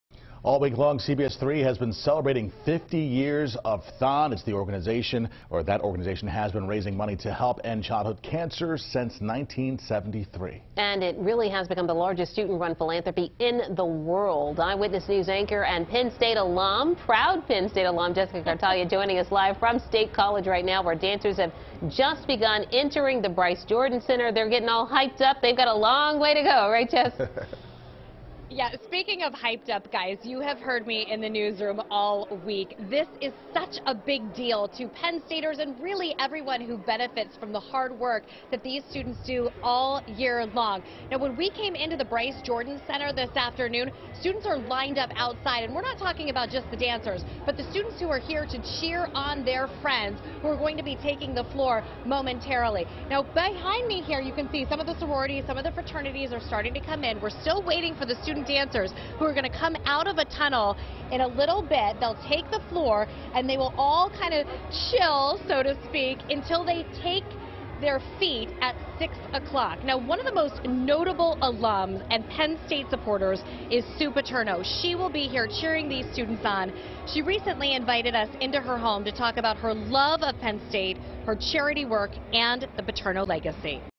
CBS3 Celebrates 50 Years Of THON: Penn State's THON Kicks Off
Jessica Kartalija reports from State College where dancers have begun entering the Bryce Jordan Center.